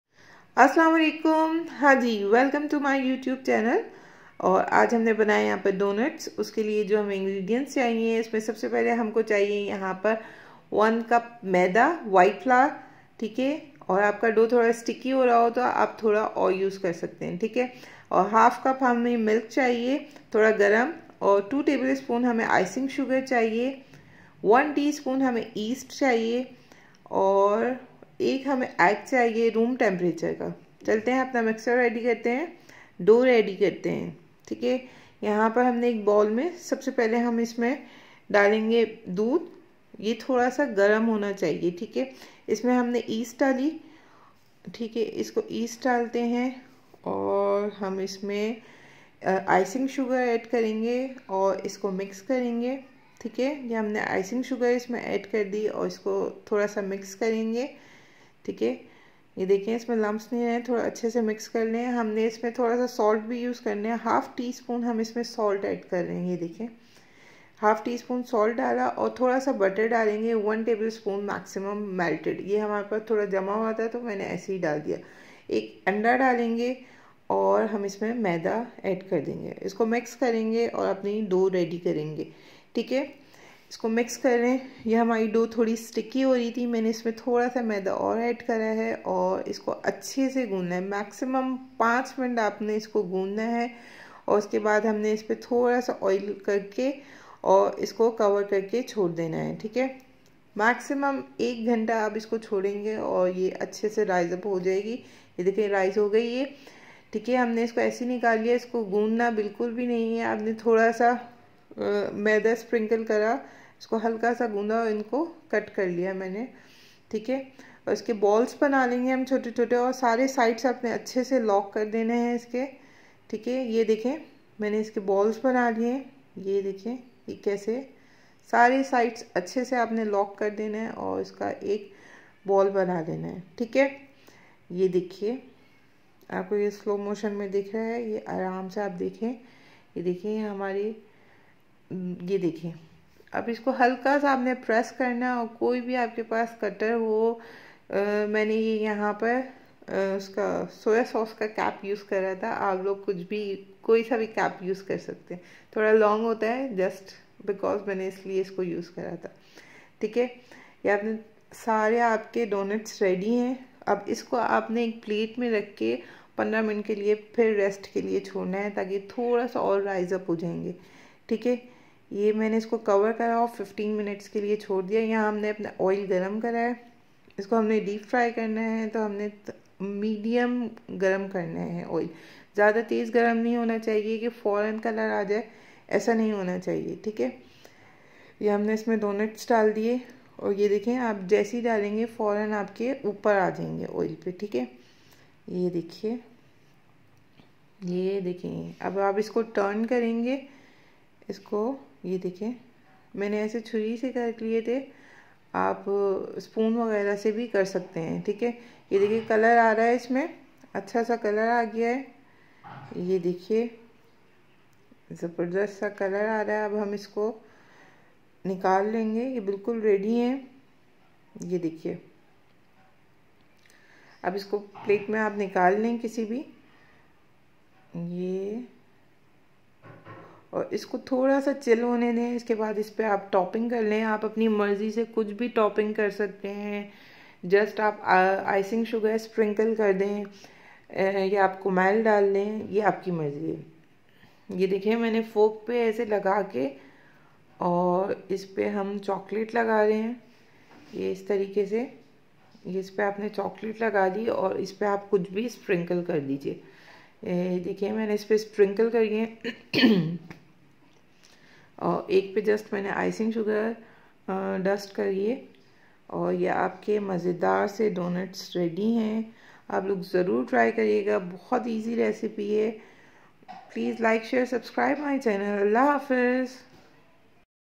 Donuts recipe/Doughnut easy recipe by san beauty and kitchen
Doughnut recipe
Easy to make donuts at home by
San beauty and kitchen
Recipe#
Egg/
White flour/1 cup
Icing sugar/2 tbsp
East/1 teaspoon
Butter/1 tablespoon
Salt/half teaspoon
Milk/half cup
Oil/for frying
Topping do u like any